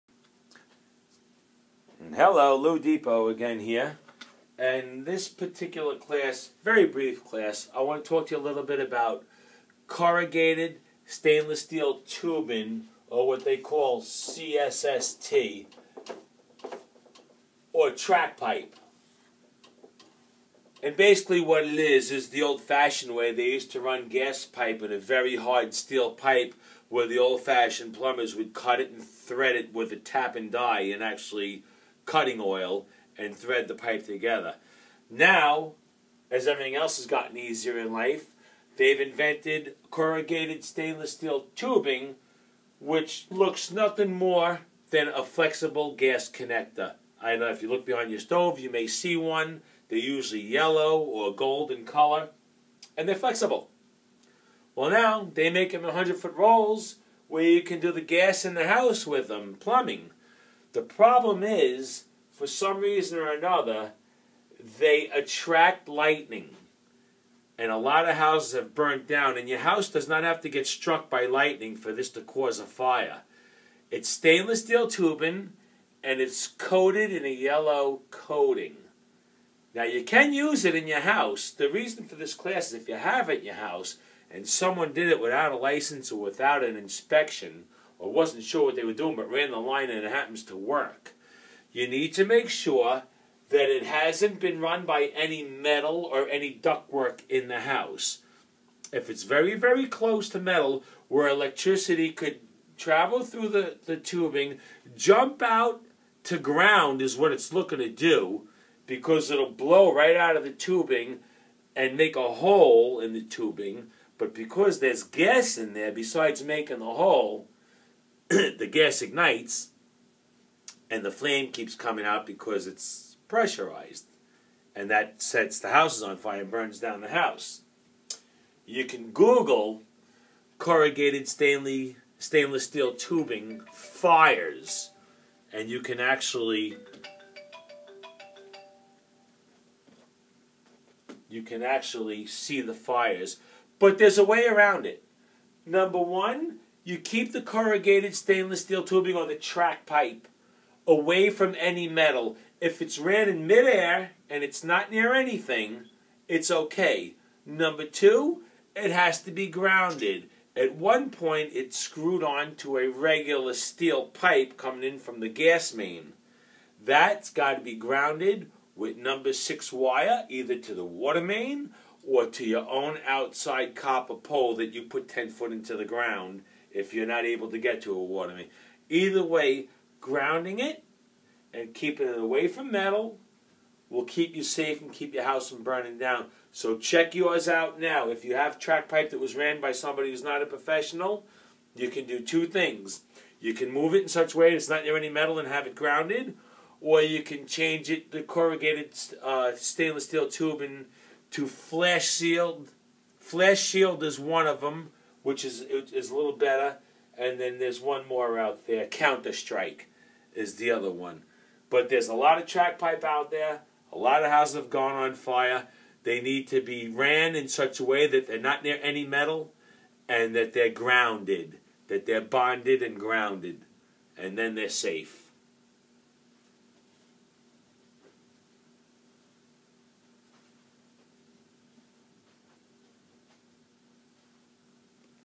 Gas track pipe, i will save your house from burning down,
Safety tips on plumbing for the new gas lines called csst or corrugated stainless steel tubing,  Like these videos? just type in loudepot in any APP store, and for .99 cents you get a lifetime of fantastic tutorials, updates for free every month..forever, thanks Loudepot!!